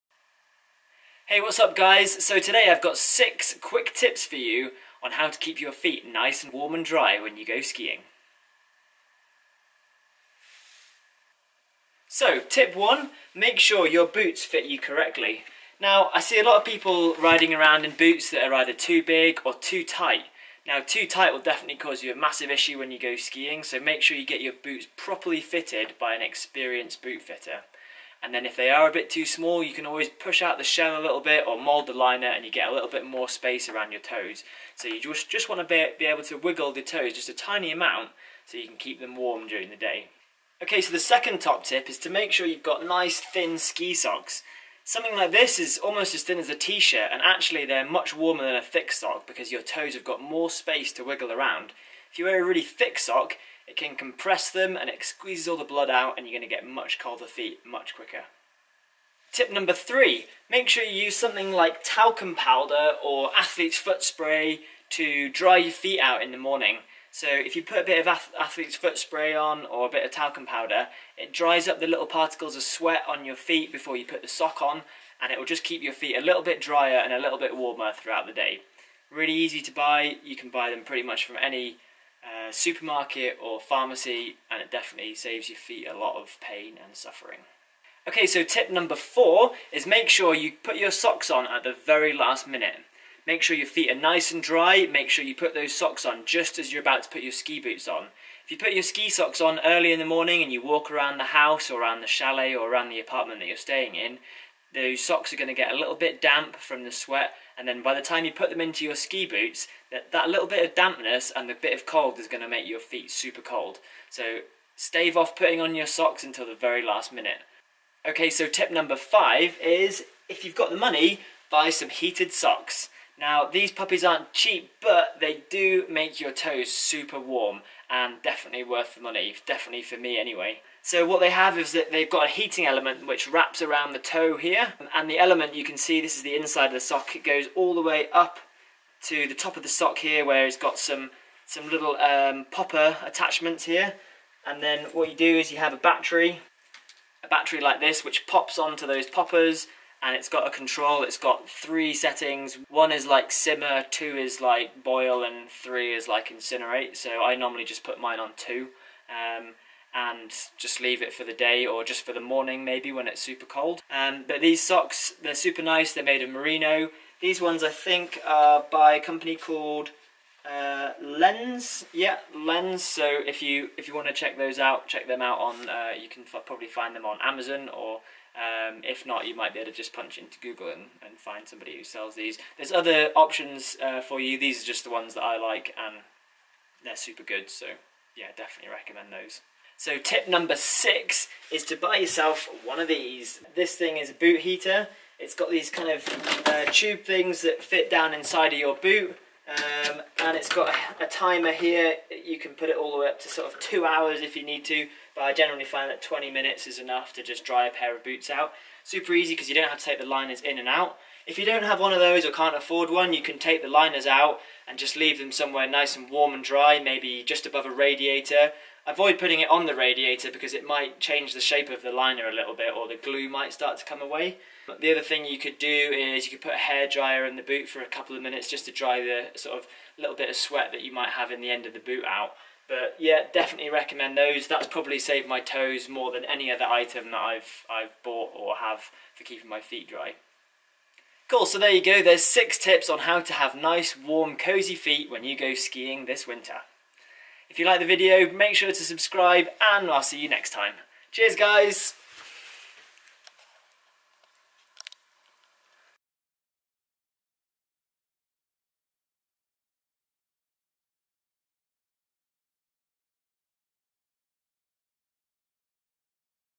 6 Tips for warmer feet when skiing // DAVE SEARLE
Here's my 6 tips for having warmer feet when you go skiing. The main idea is keeping your feet dry before you head out.

to book me as a mountain guide or for other articles, trips reports and more.